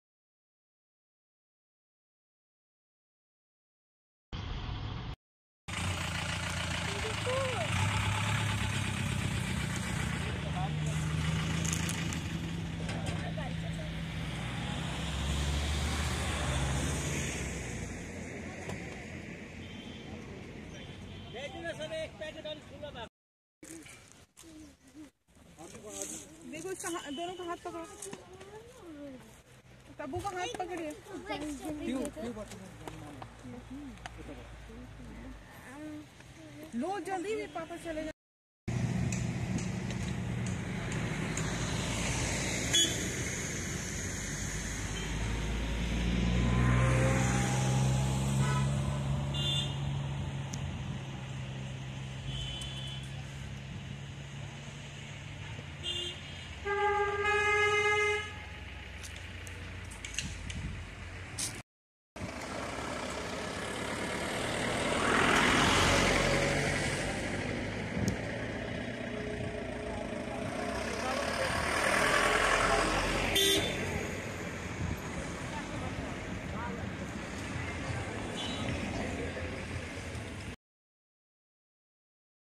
Organic Strawberry Cultivation at North Bengal, West Bengal, India
Cultivation of Organic Strawberry at Rohini on the way to Darjeeling, the famous hill station of North Bengal. Entire field is covered with polythene so that Strawberries do not come in contact with soil and thus preventing bacterial and fungus infection to the fruits.